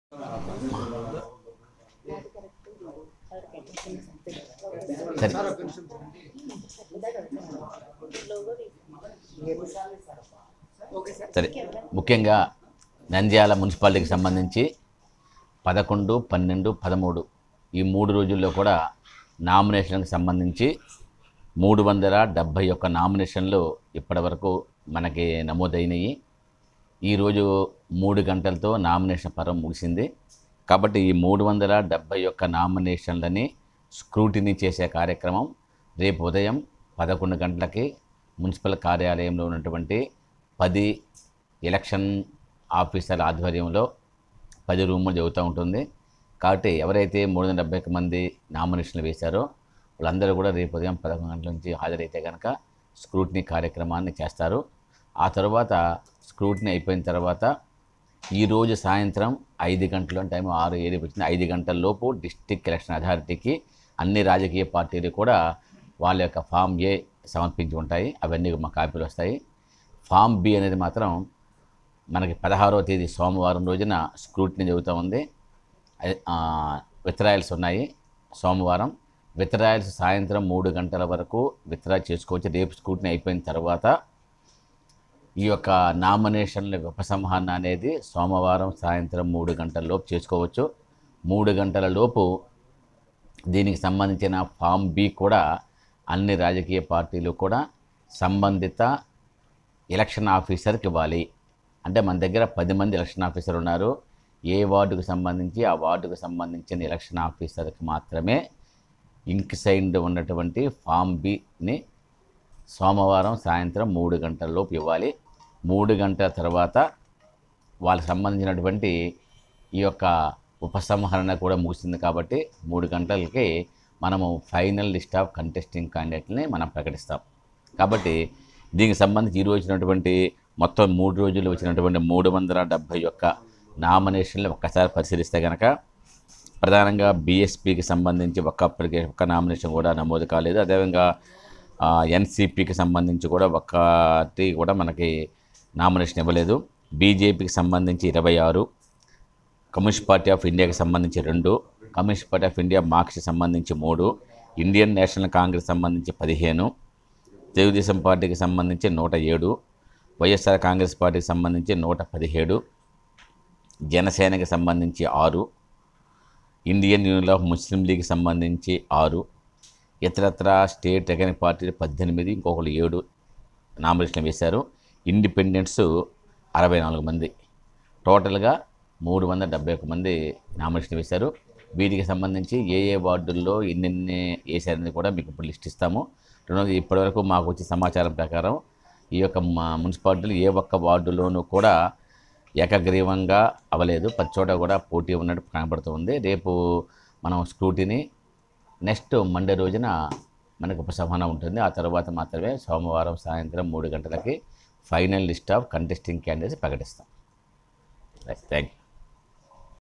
నంద్యాల మున్సిపాలిటీ నామినేషన్ వివరాలు తెలిపిన కమిషనర్
Nandyal news9--- అనగా తేదీ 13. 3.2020 నంద్యాల పురపాలక సంఘం నందు సాధారణ ఎన్నికల్లో భాగంగా  335 నామినేషన్లు దాఖలు కాబడినది. తేదీ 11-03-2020 న 3 నామినేషన్లు, తేదీ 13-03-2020 న 33 నామినేషన్ల  తో కలిపి మొత్తం 371 నామినేషన్ దాఖలు చేసి ఉన్నారు. అందులో BJP - 26, CPI-2, CPM -3, INC -15, TDP-107 , YSRCP- 119 , జనసేన  - 6, SDPI -18  , AIMIM - 7, IUML- 6, INDEPENDENTS -62.కర్నూలు జిల్లా నంద్యాల మున్సిపాలిటీ పరిధిలోని 42 వార్డులకు గాను రెండోవ రోజుకు 371 నామినేషన్ లు దాఖలు అయ్యాయి..

వైసీపీ తరపున -119 ,
టీడీపీ తరుపున - 107,
బీజేపీ తరపున - 25,
సిపిఐ తరపున - 2,
స్వతంత్ర అభ్యర్ధులు గా - 62,

మొత్తం నామినేషన్ లు 371 దాఖలు అయినట్లు తెలిపిన మున్సిపల్ కమిషనర్ వెంకట కృష్ణుడు..